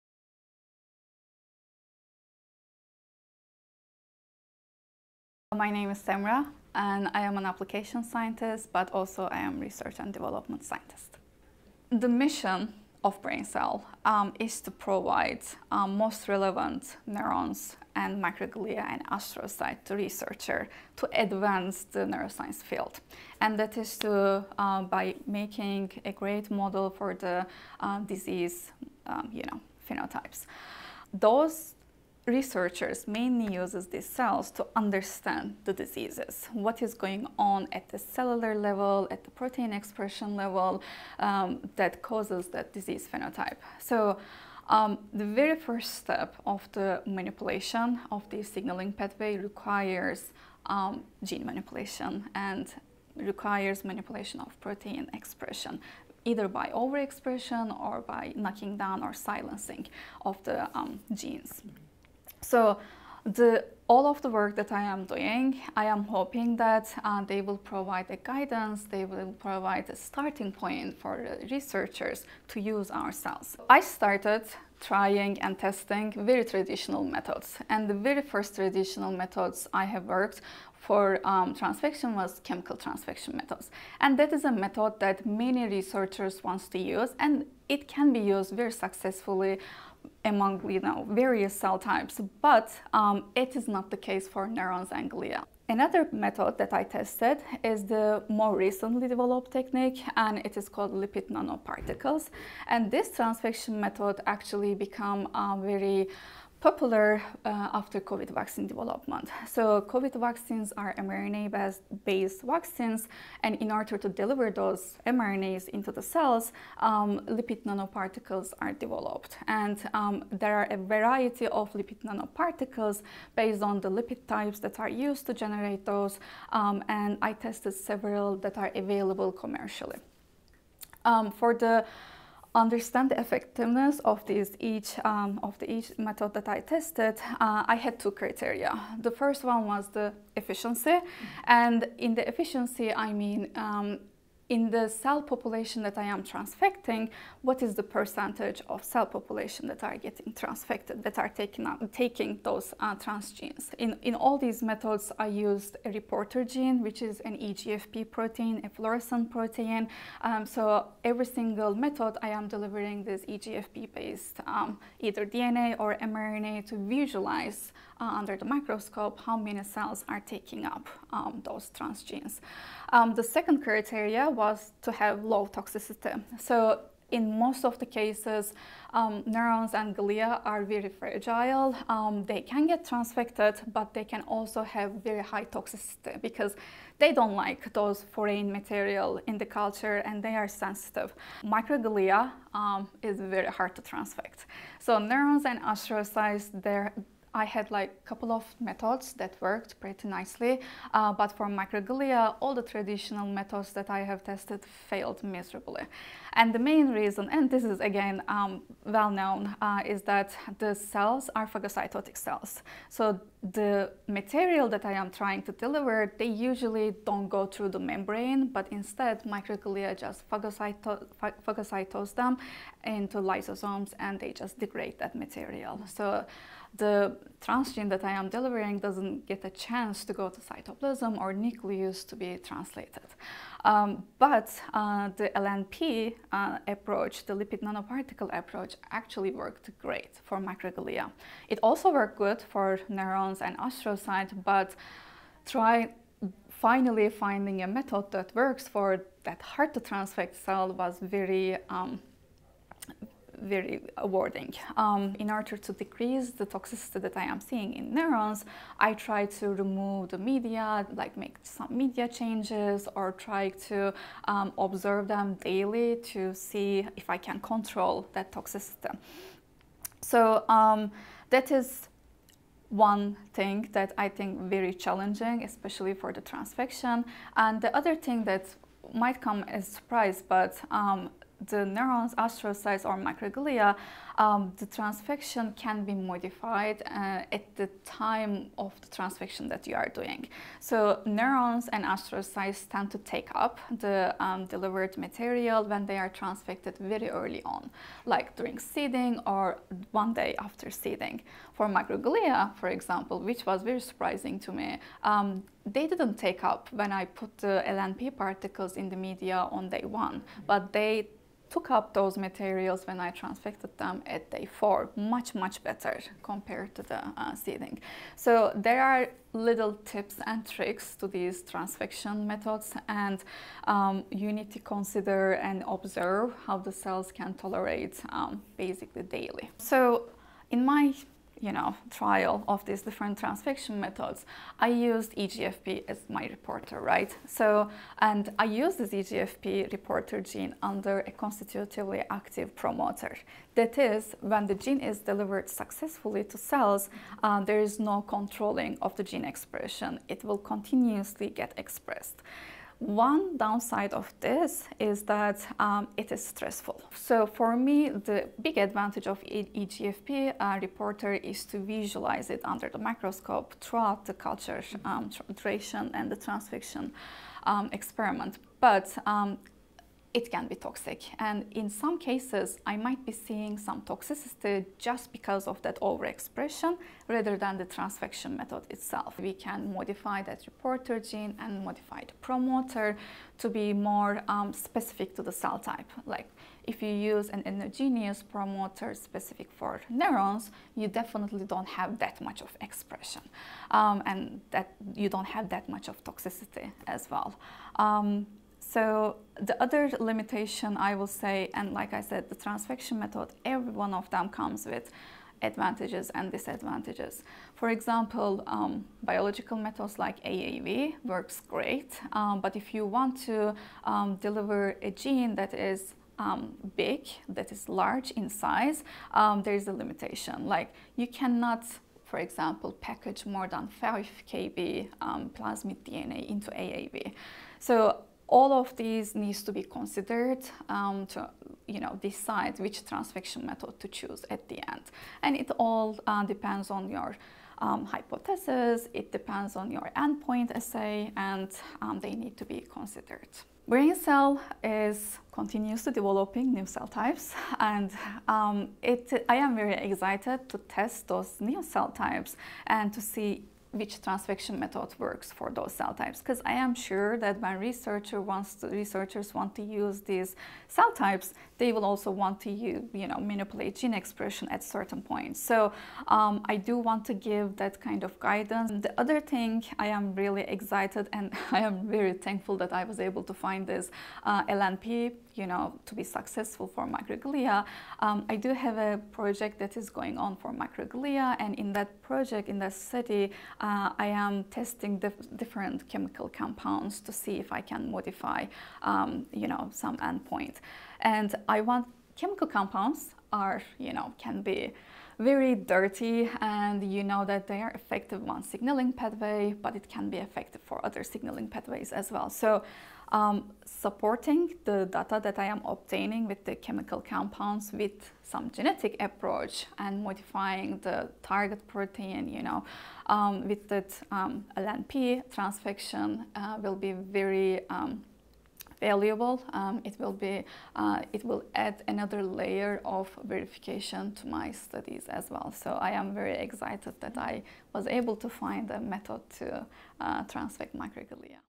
BrainXell Semra Sahin Transfections of iPSC derived Neurons and Glia
Watch Semra Sahin, Applications and R&D Scientist at BrainXell, Inc. discuss the transfection of iPSC-derived neurons and glia. Learn about the techniques and applications that are advancing neuroscience research.